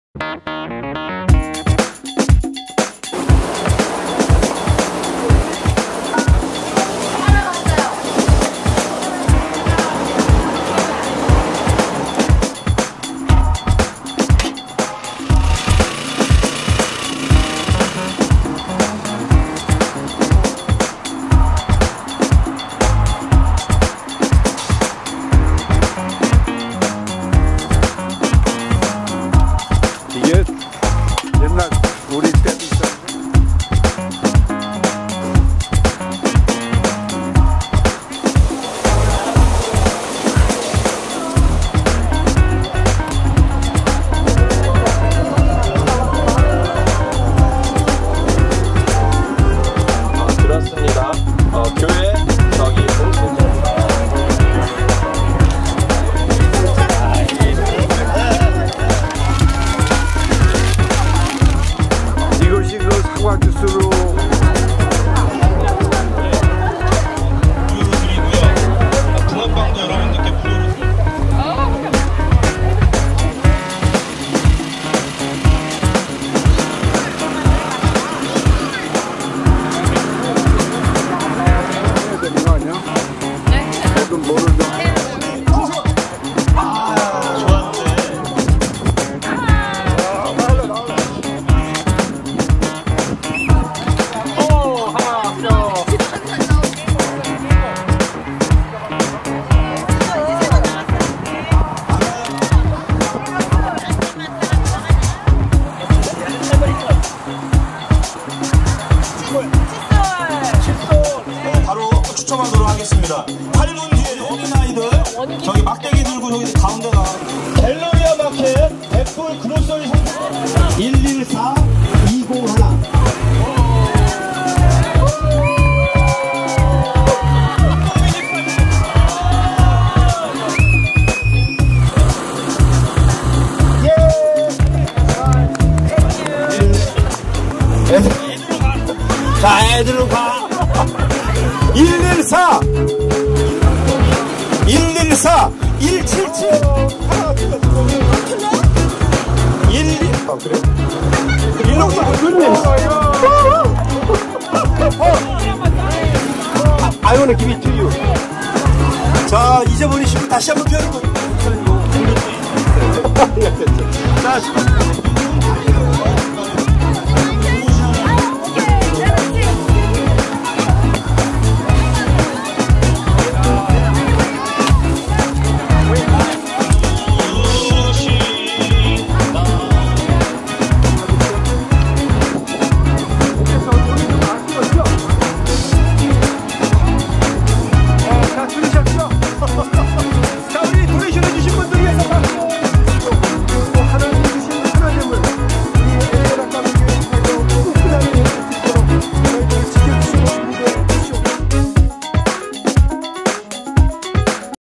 Happy Church Picnic.mp4
May is green. May is hope. May is youth.
We are rejoyce with God's grace, share our love in Christ.